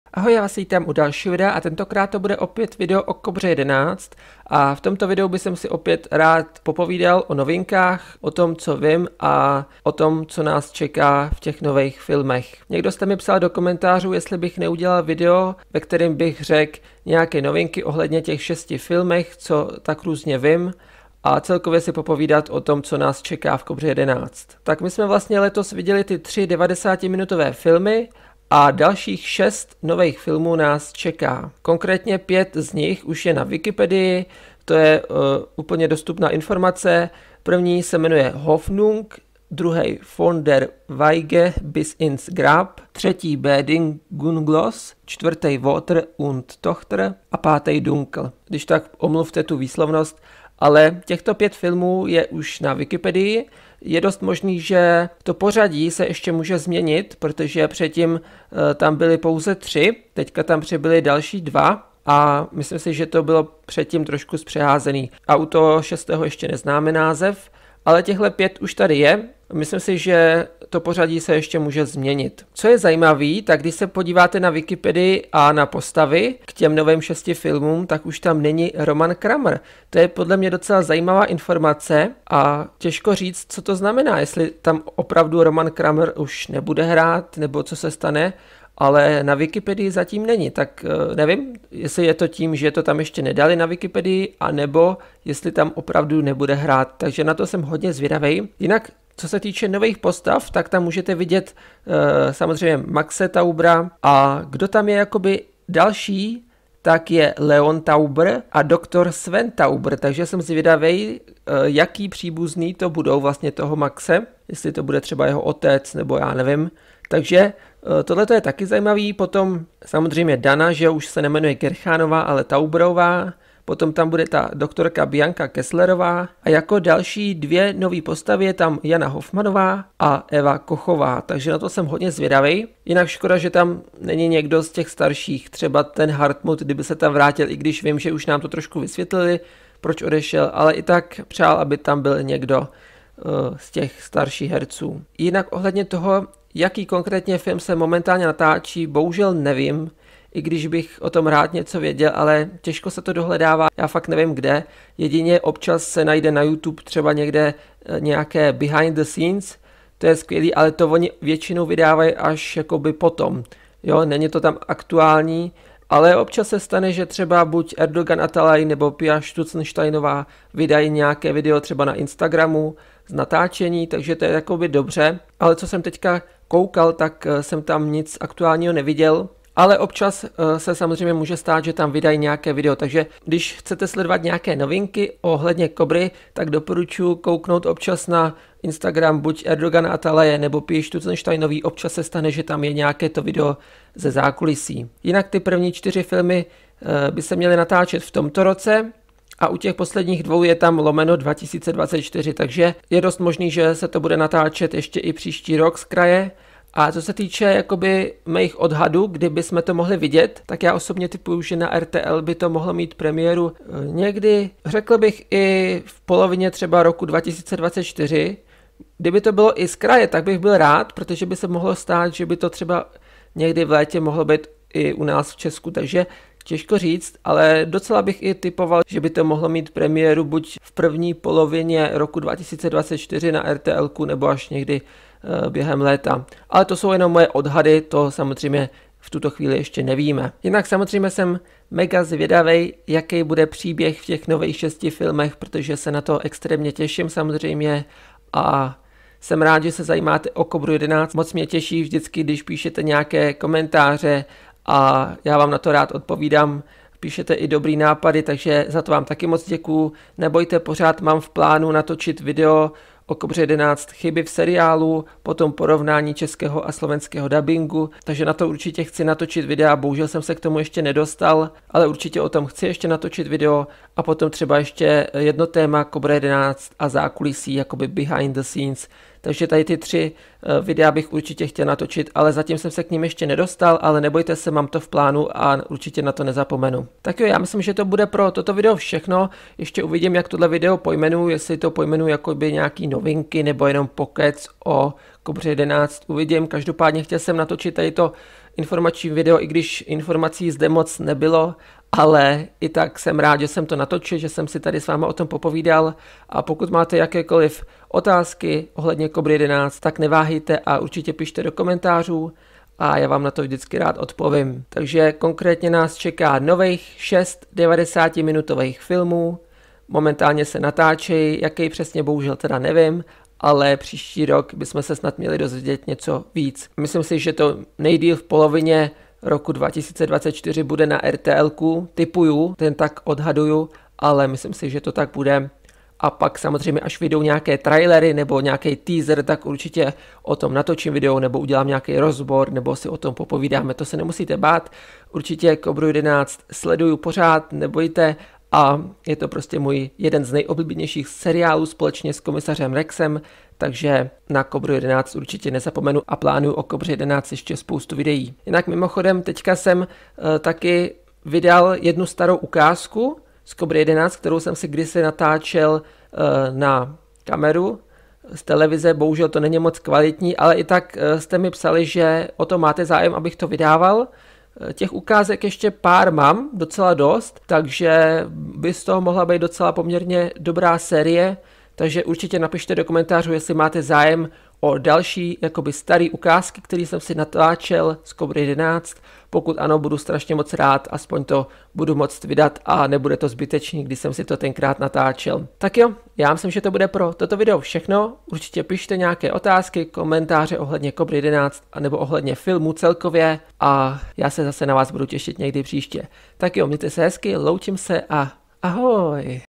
Pokec o Kobře 11 - Novinky, spekulace o nových 6 filmech!
Vítejte u dalšího videa z Kobry 11, dnes si popovídáme o nových filmech a dalších věcech.